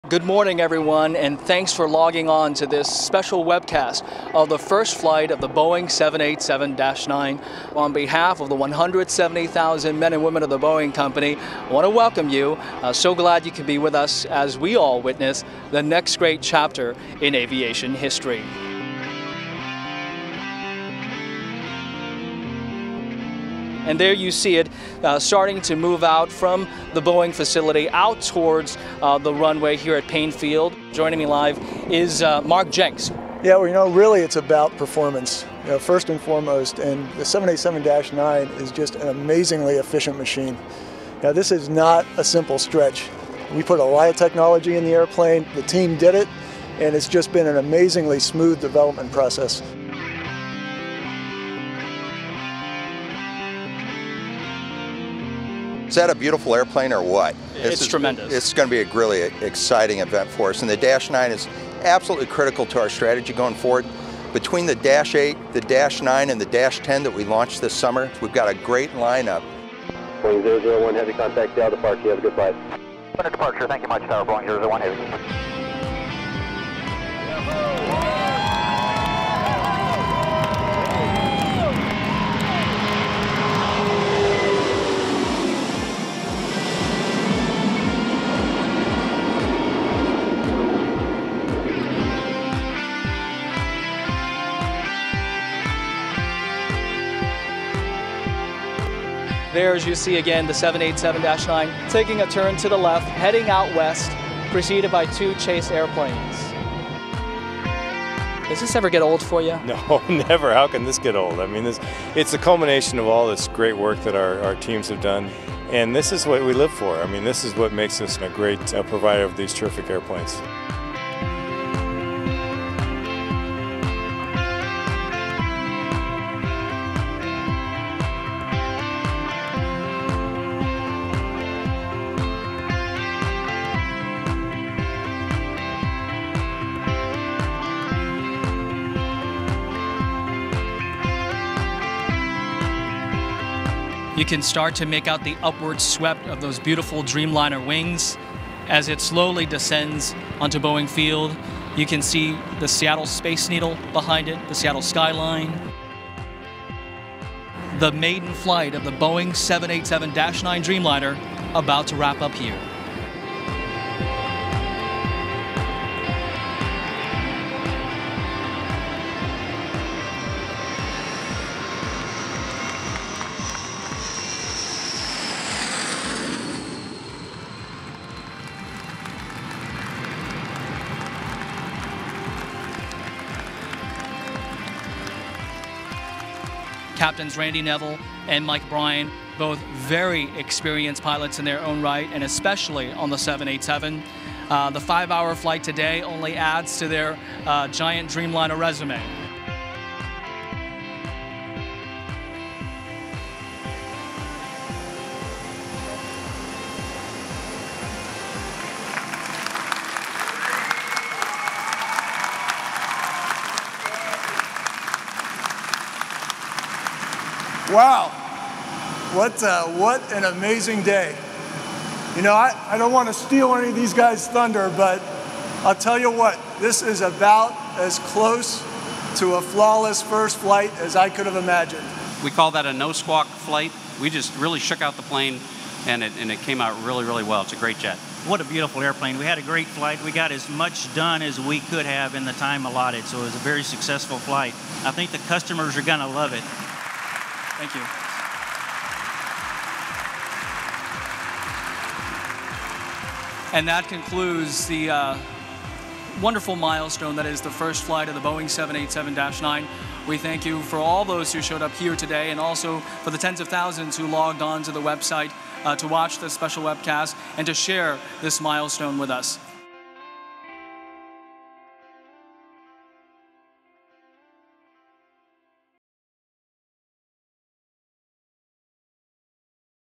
Boeing's 787-9 Dreamliner First Flight "On Cloud Nine"
A major milestone for the 787 program. The New 787-9 Dreamliner completed it maiden voyage on September 17, 2013. The second member of the 787 family, the 787-9 is 20 feet longer and carries 40 more passengers than the 787-8.  On its first flight the 787-9 flew flawlessly for over 5 hours. A truly historic event.